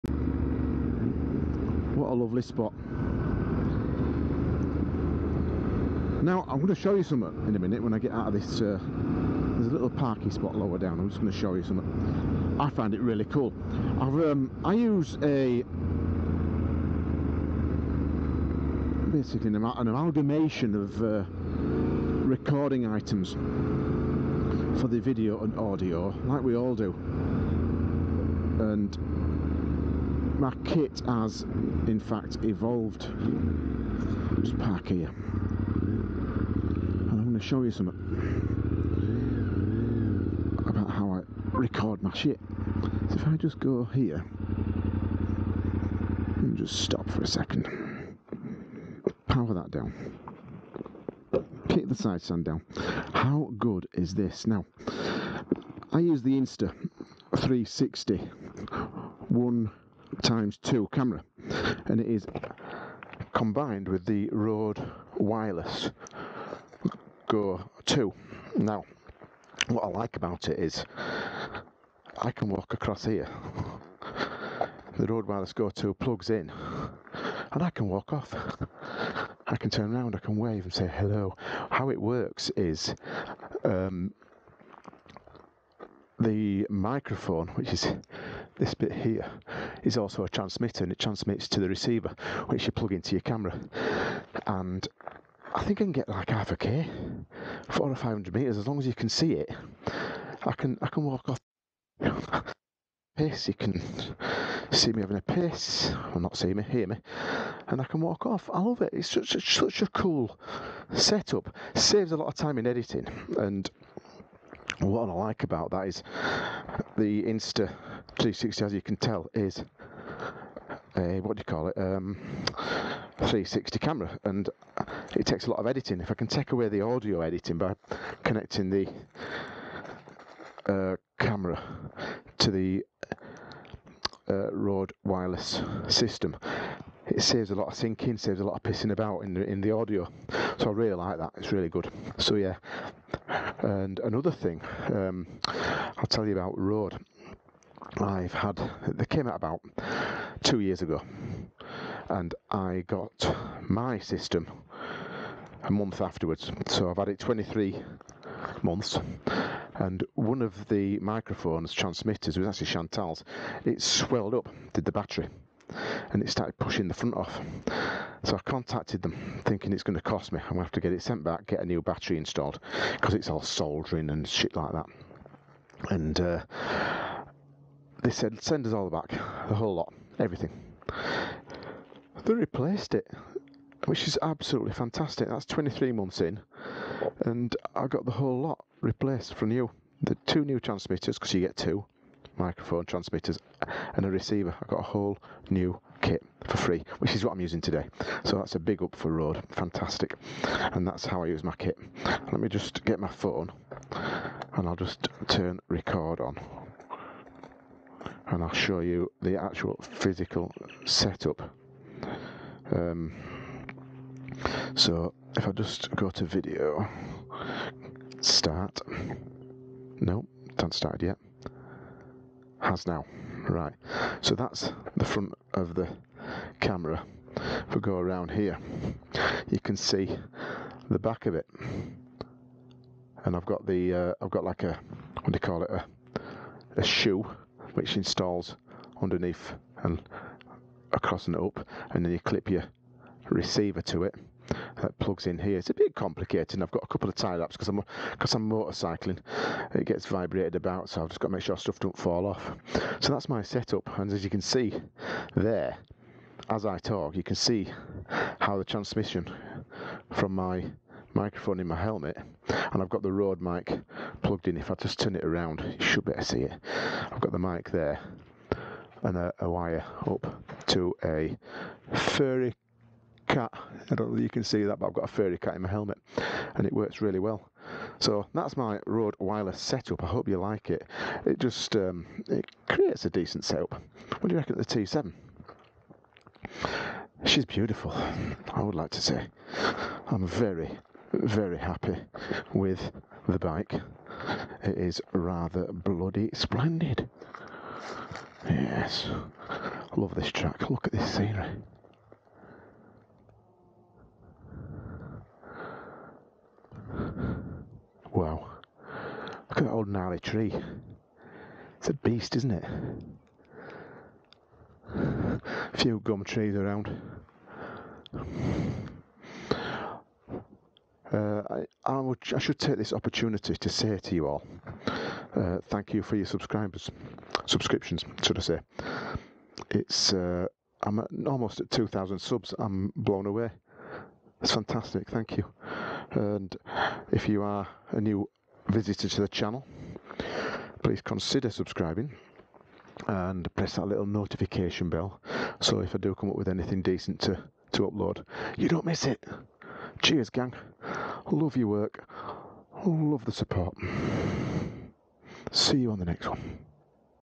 INSTA360 ONE X2 and RODE WIRELESS GOII MOTO COMBO
My ACE Insta360 & Rode Wireless video and audio motorcycling set up.  Check it out, see just how cool and versatile it is for recording and syncing video to 'separately' recorded audio.  I think at some point I'll be looking towards the Insta360x3 with the crazy new features but as you can see (and hear) I'm still enjoying superb features with the x2.  Brilliant stuff.